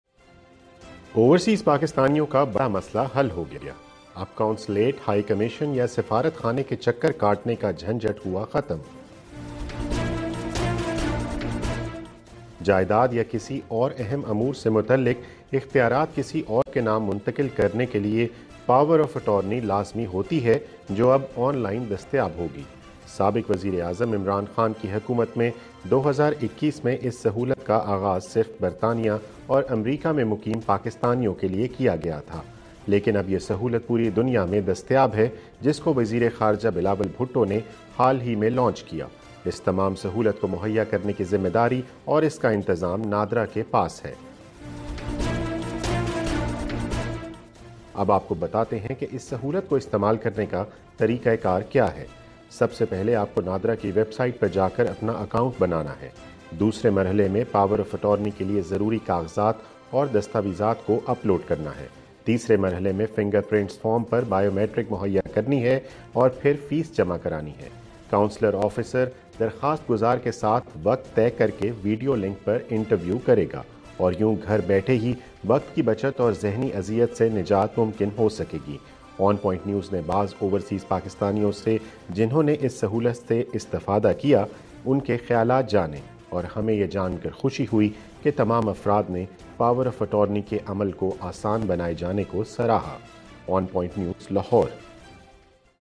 Online Power of Attorney for overseas Pakistanis.
Pakistan government has started a much needed service for overseas Pakistanis. Now they can get a power of attorney online while being in any country across the Globe.